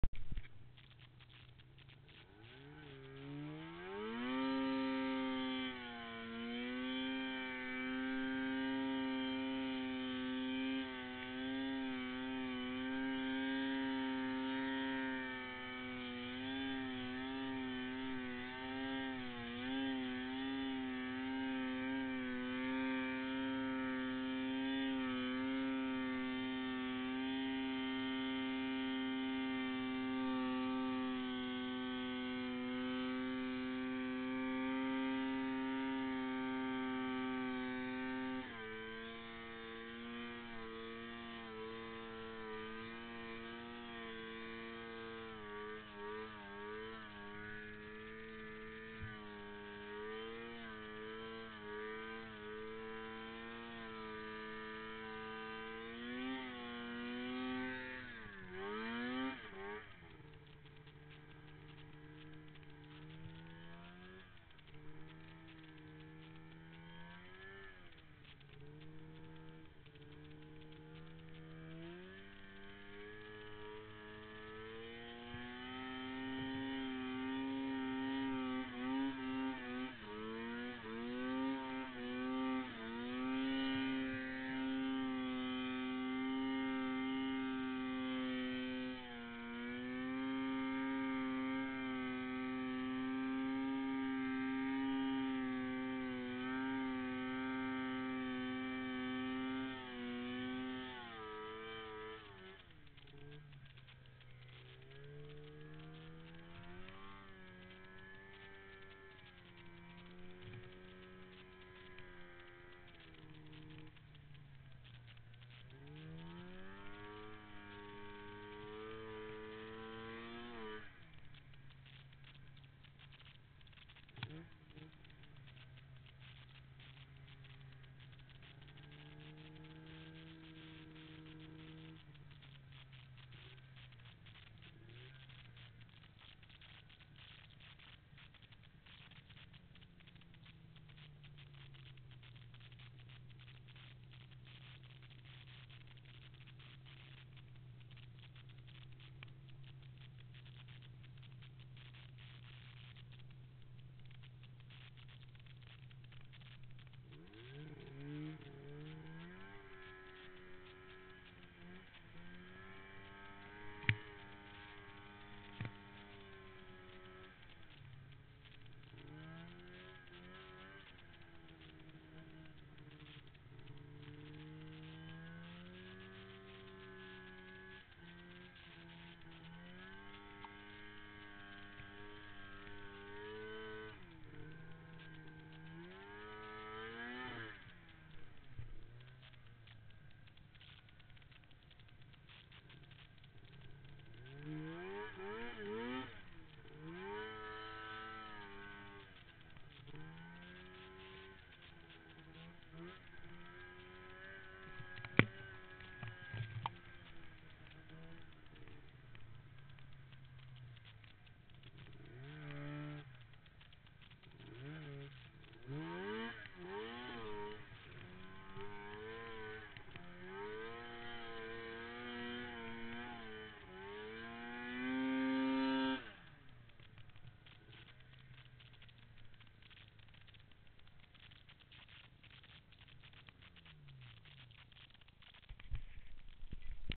Mt Baker pow 2-28-10
Carving some turns and a little side hilling up at Baker on a pow day in the sun